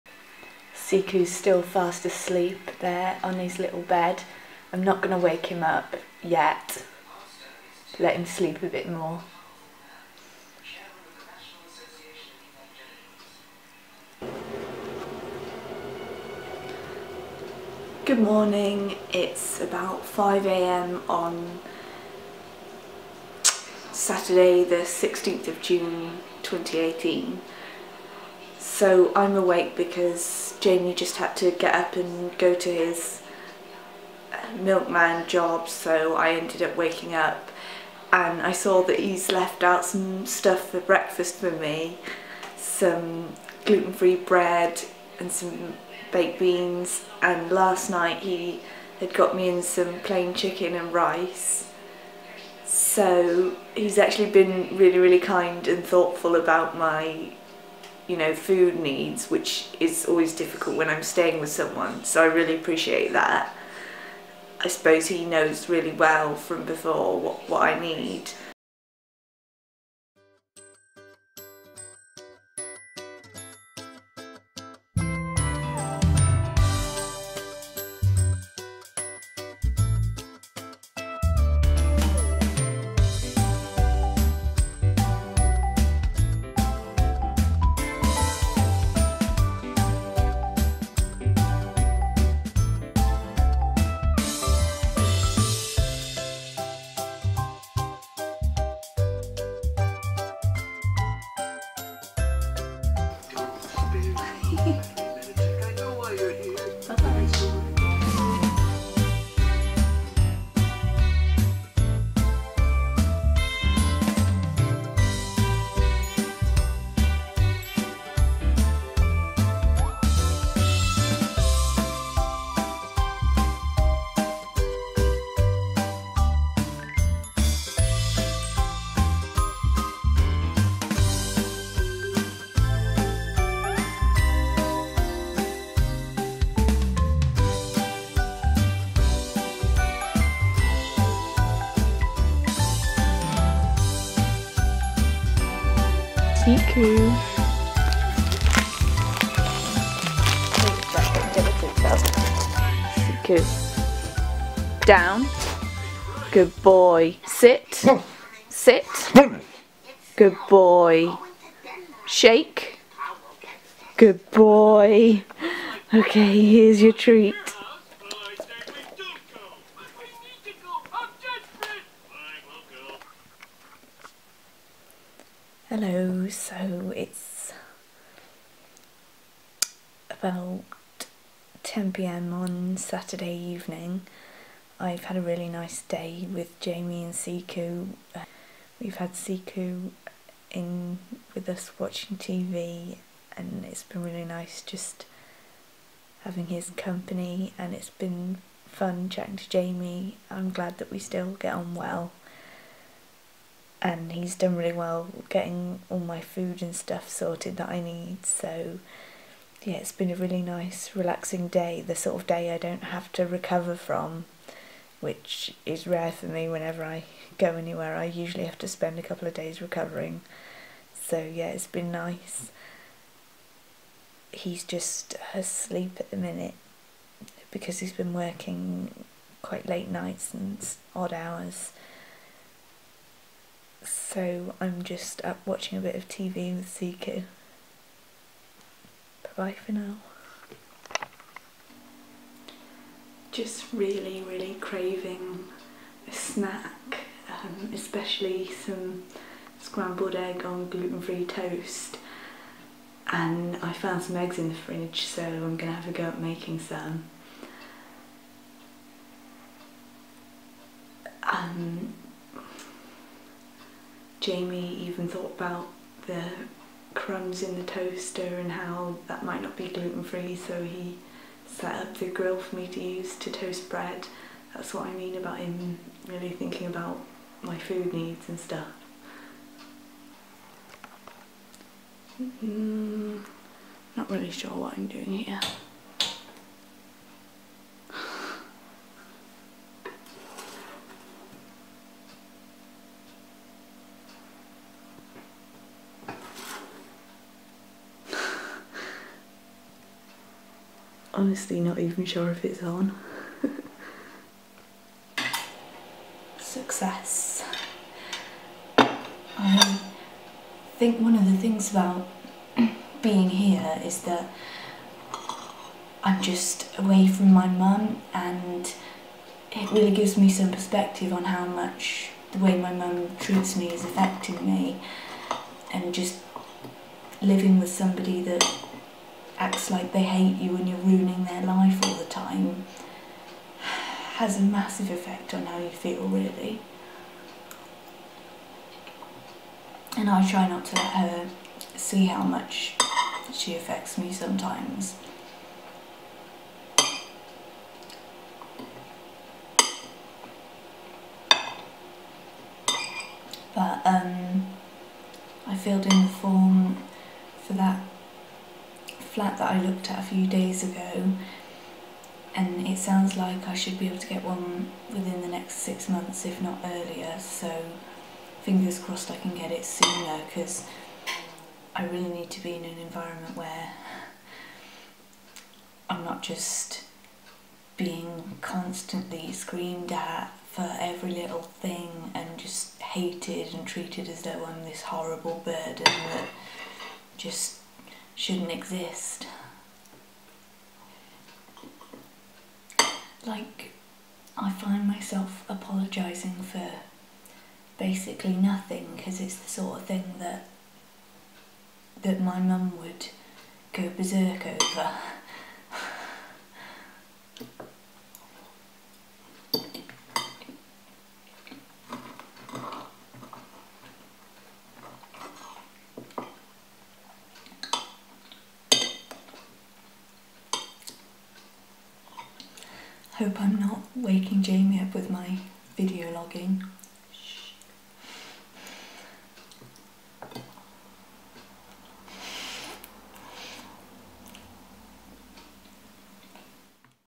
Hatty's Vlog - Dog Day [17.06.18]
Day 2 in Nottingham, I spend a relaxing day with Jamie and Siku the Alaskan Malamute. I also talk a little about how we're handling my food intolerance issues during the trip, and I share my thoughts and plans in relation to my current living situation.